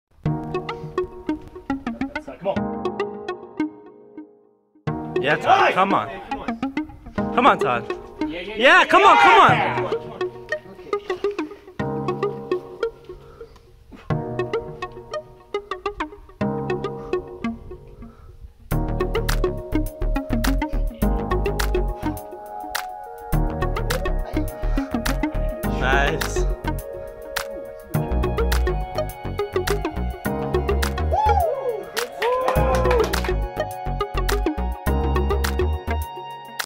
DDD (V7) - Hueco Tanks, TX
No gaston/no heel hook beta for DDD.